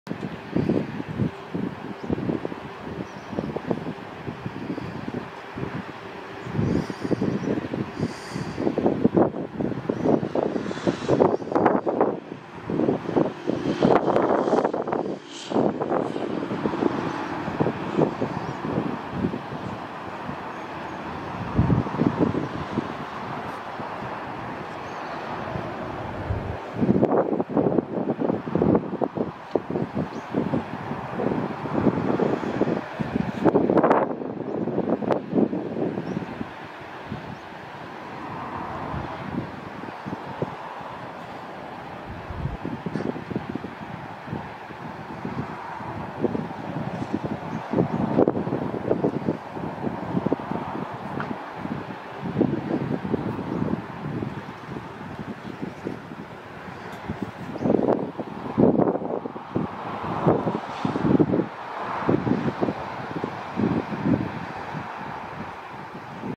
a minute of OBO entering Princess Royal Harbour
standing on Marine Dr at the Rotary Lookout, two tugs escort the OBO (Oil/Bulk/Ore) carrier in